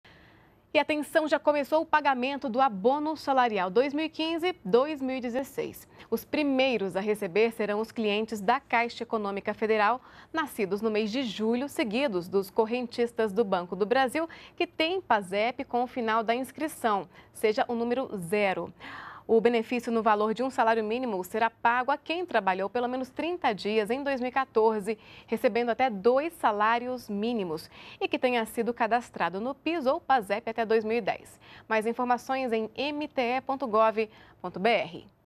Começa pagamento do Abono Salarial 2015/2016
NBR NOTÍCIAS - 16.07.15: Começou o pagamento do Abono Salarial 2015/2016. Os primeiros trabalhadores a receber serão os correntistas da Caixa Econômica Federal nascidos no mês de julho. Já os clientes do Banco do Brasil (BB) começam a resgatar o valor do PASEP nesta sexta-feira (17). Tem direito ao benefício de um salário mínimo quem trabalhou por, pelo menos, 30 dias no ano-base 2014, recebendo até dois salários mínimos e que tenha sido cadastrado, até 2010, no PIS, para trabalhadores celetistas, ou no PASEP, para servidores públicos.